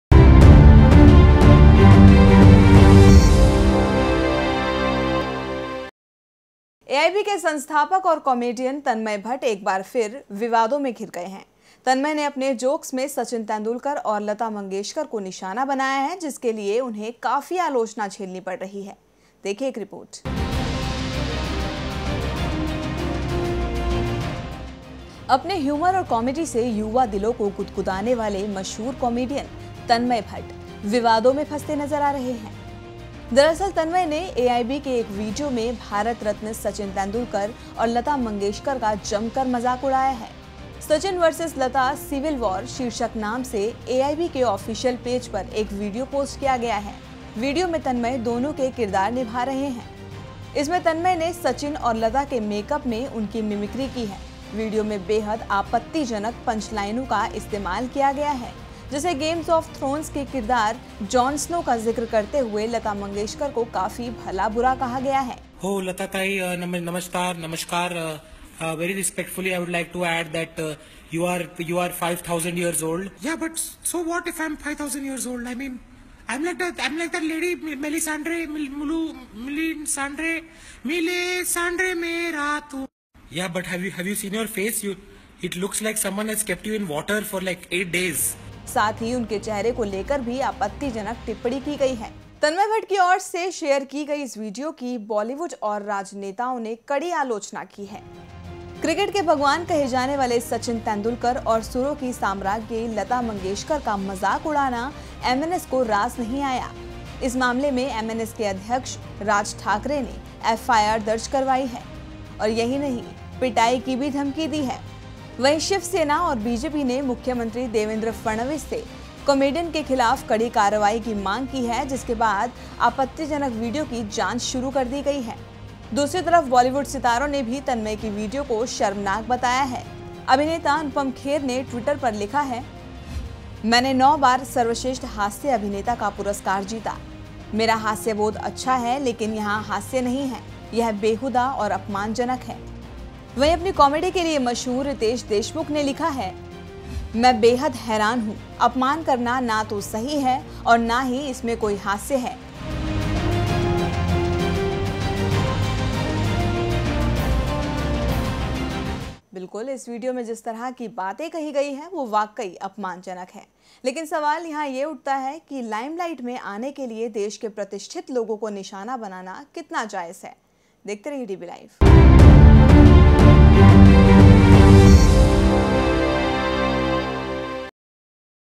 DBLIVE | 30 May 2016 | MNS files FIR against AIB's Tanmay Bhat for video mocking
एआईबी के संस्थापक और कॉमेडियन तन्मय भट्ट एक बार फिर विवादों में है..तन्मय ने अपने जोक्स में सचिन तेंदुलकर और लता मंगेशकर को निशाना बनाया है..जिसके लिए उन्हें काफी आलोचना झेलनी पड़ी रही है।